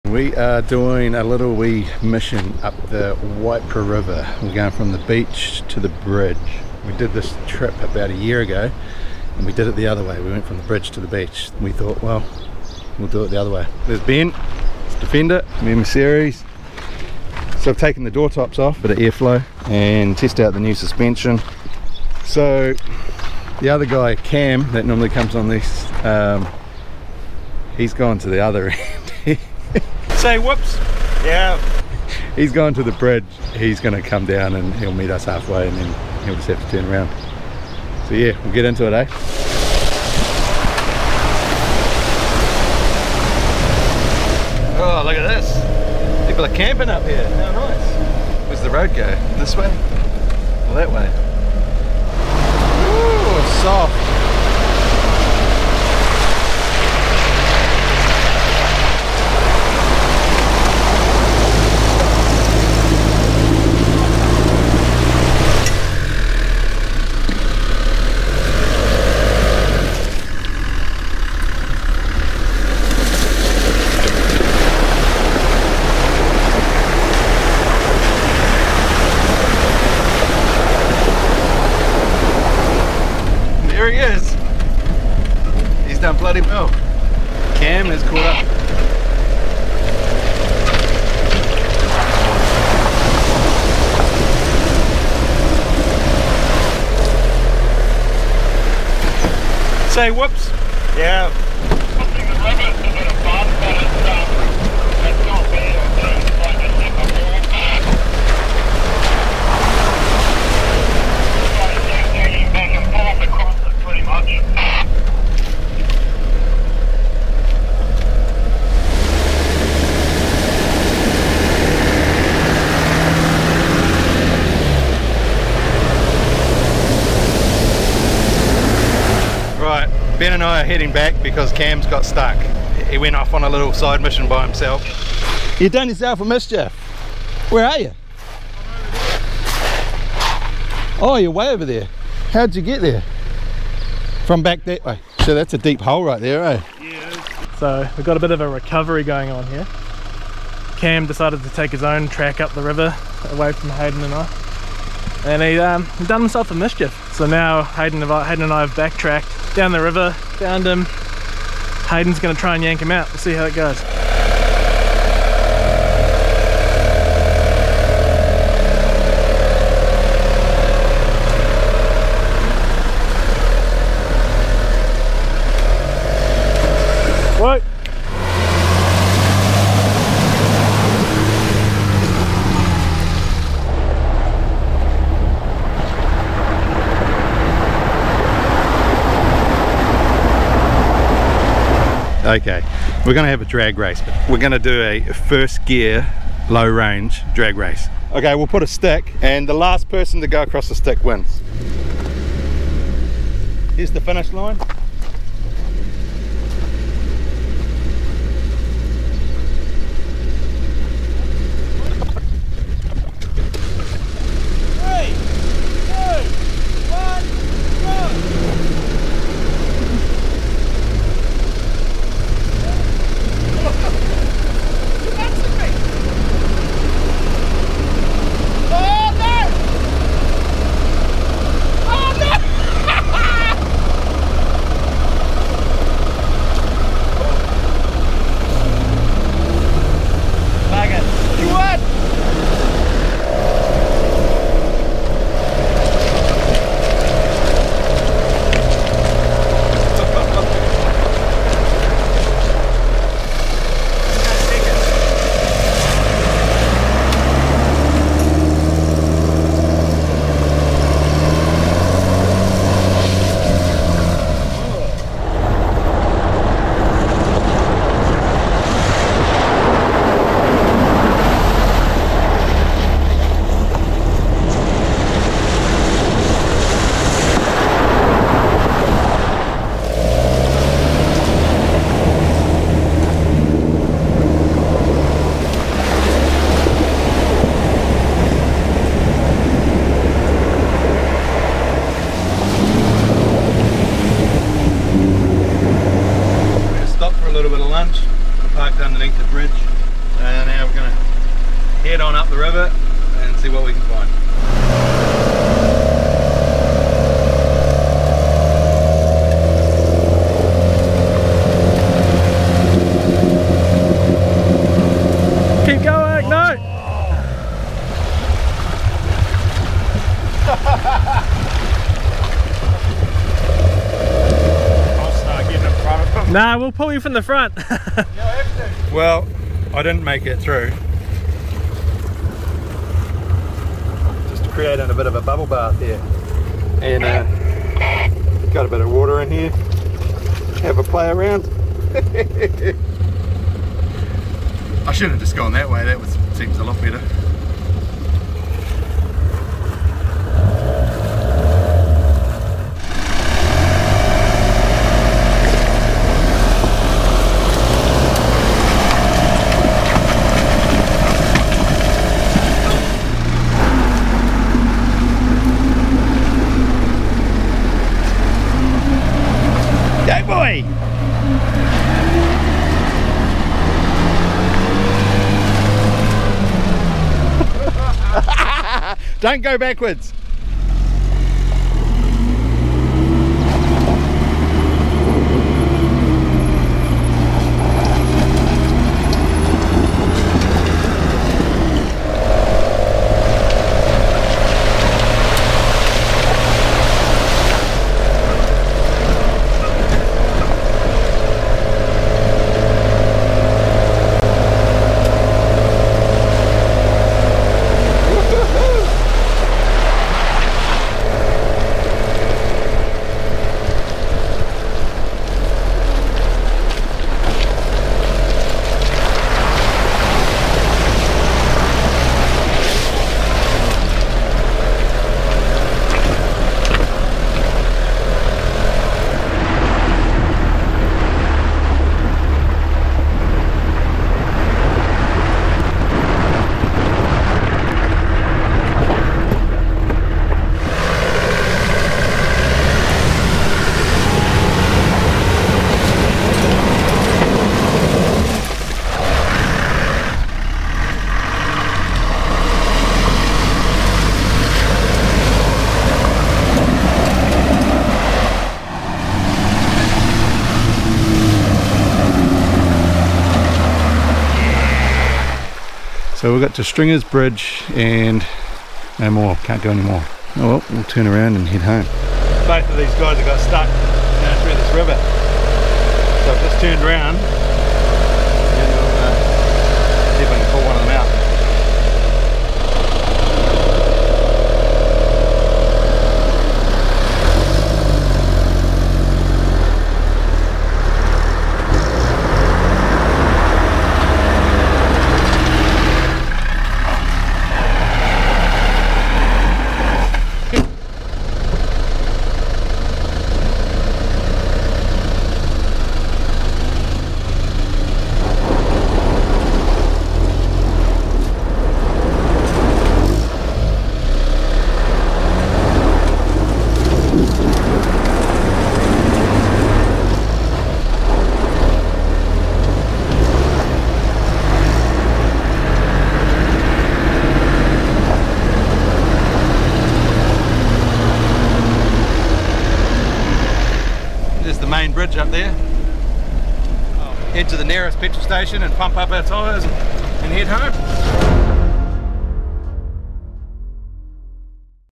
New Zealand Off-Roading at it's Best: Classic Land Rovers Take on the Waipara River
Another fantastic day out in our Land Rovers, exploring the Waipara River in Canterbury, New Zealand
We went from the beach all the way to stringers bridge then back to the state highway bridge.

Discover your next track with this website - a treasure trove of adventures waiting for you to explore.